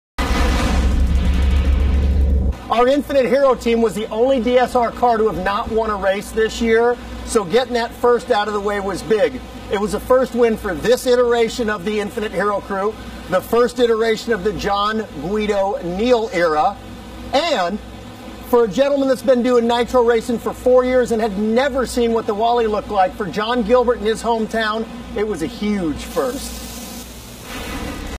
SPEED - Cool piece w/Jack & Crew Member, John Gilbert
Big thanks to SPEED for this cool piece with Jack and crew member, John Gilbert!